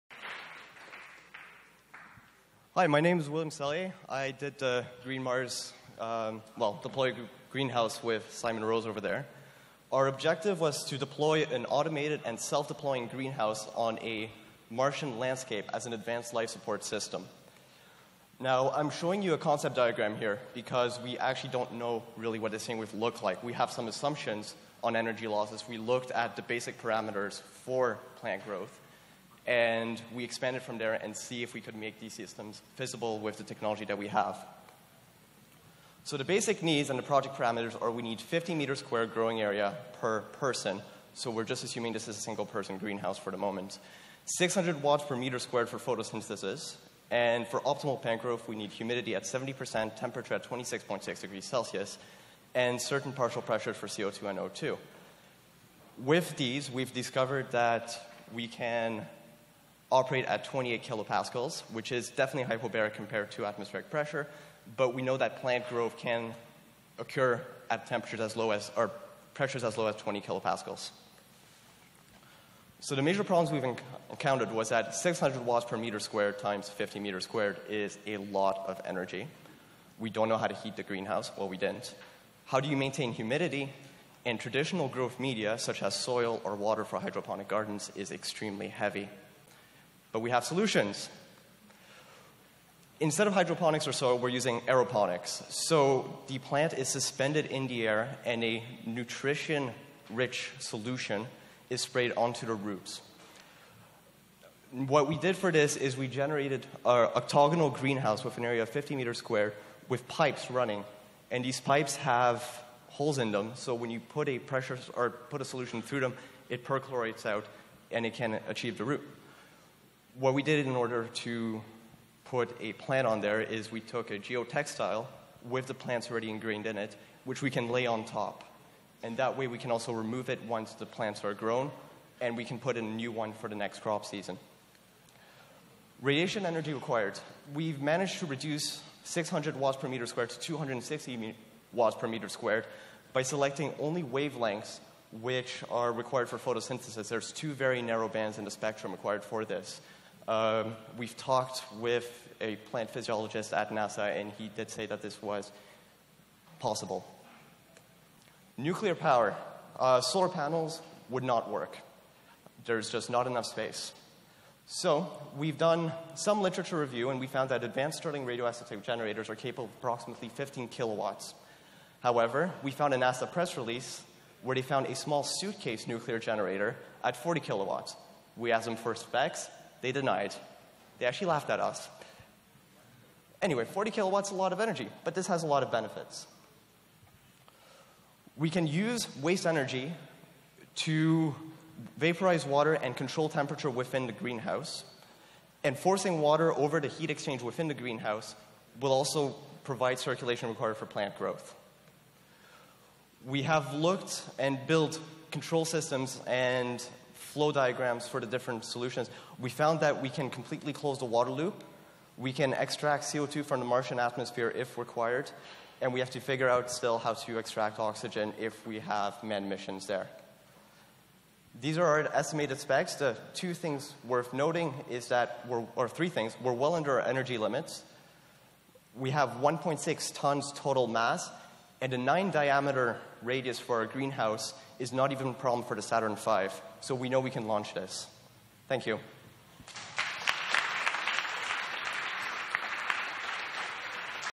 Green Mars — NASA SpaceAppsTO 2013
This project is solving the Deployable Greenhouse challenge.

Automated and self-deploying greenhouse on martian landscape as an advance life support system.

Resources

DEVELOPMENT OF A CONTROL ALGORITHM FOR A DYNAMIC GAS MIXING SYSTEM
FIRST OPERATION OF A 12 kWe STIRLING POWER CONVERSION UNIT FOR FISSION POWER TECHNOLOGY DEMONSTRATIO
GENETIC AND PHYSIOLOGICAL DAMAGE INDUCED BY COSMIC RADIATION ON DRY PLANT SEEDS DURING SPACE FLIGHT
PLANTS FOR HUMAN LIFE SUPPORT IN SPACE: FROM MYERS TO MARS